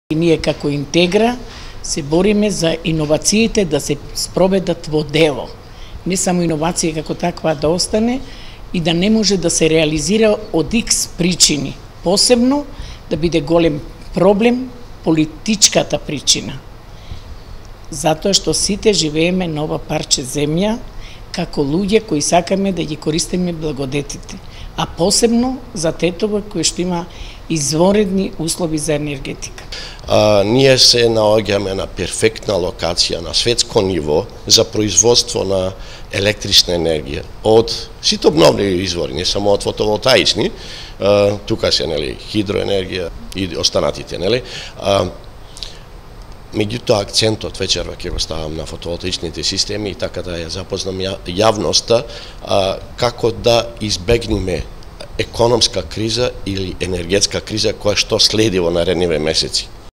Интегра со второ предавање во серија на тема енергетика
Предавање на тема енергетика и претстојната енергетска криза, вчера одржа м-р Зоран Митрески во рамки на серијата предавања на различни теми што ги организира партијата Интегра.